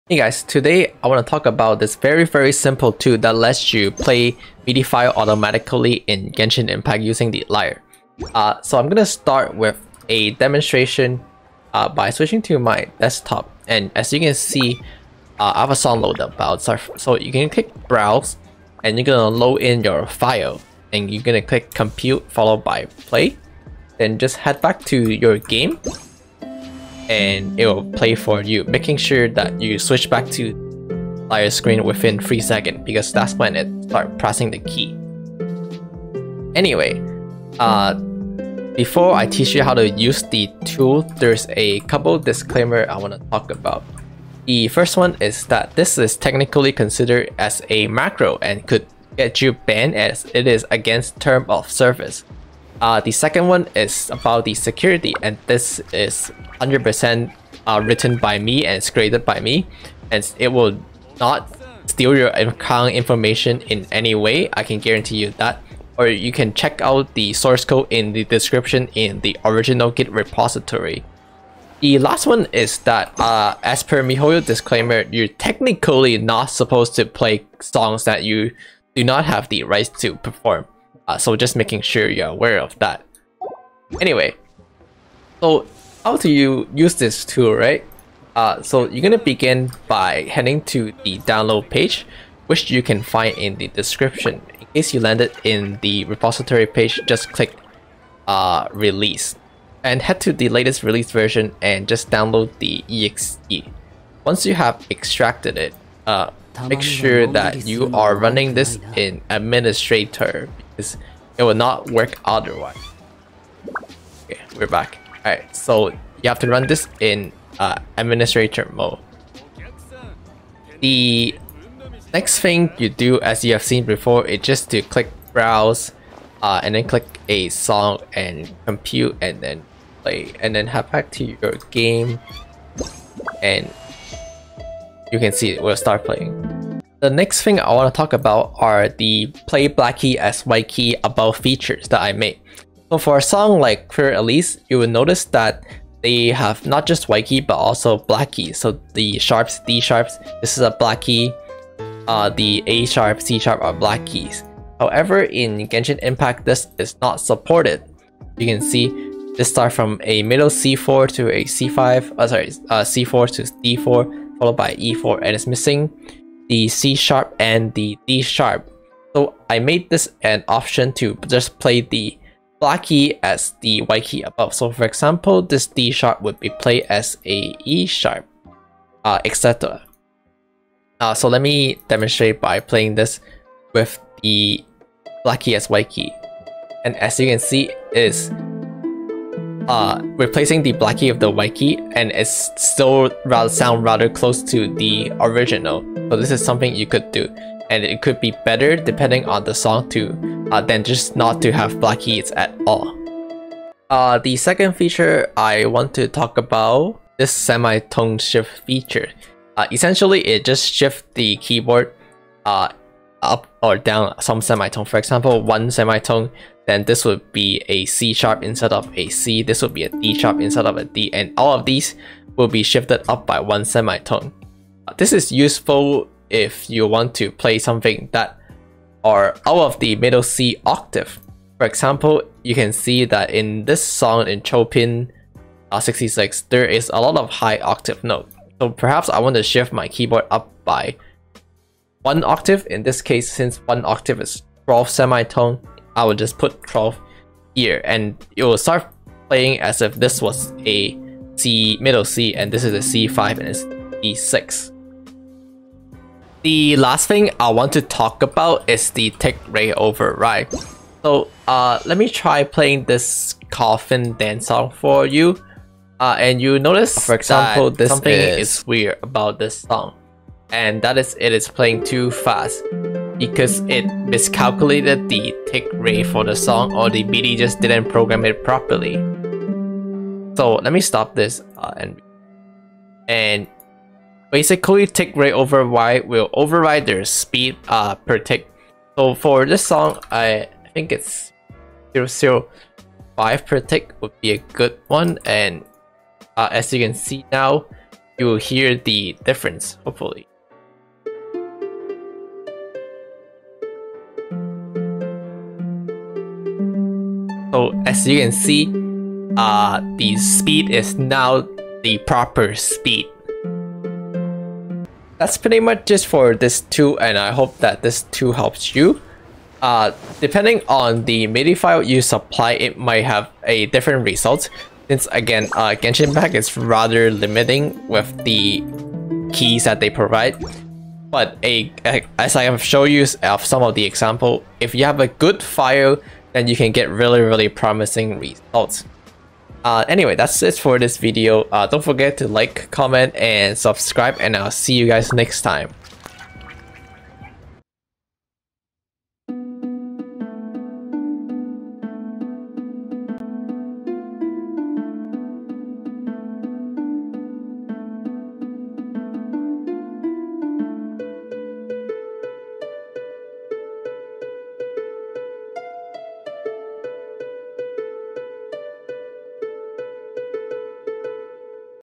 ONE click AUTOPLAY your favorite song with the Lyre using this SIMPLE tool
Where to find midi file:
that uh..well google can probably help

Warning:
just like every program in the world , this is not a bug free program(probably)

TS:
00:00 demo
00:44 DISCLAIMER
01:30 set up
02:17 Play blackkey as whitekey
03:24 Semitone shift
04:15 Tick rate override
05:14 Final word